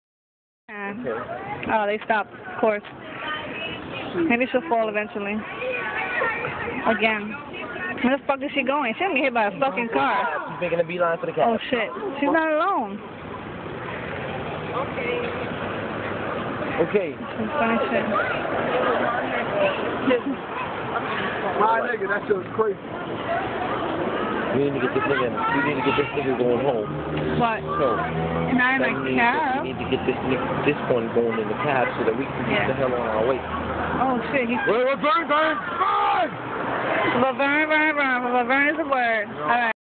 DRUNKIN WHOREDERVES AFTER A NIGHT OF CLUBING LOL
THESE CHICKS WAS WILD IN THE CLUB LOL STLL PICKING UP THEIR SKIRTS  & DRESSES OUTSIDE SITTING ON THE GROUND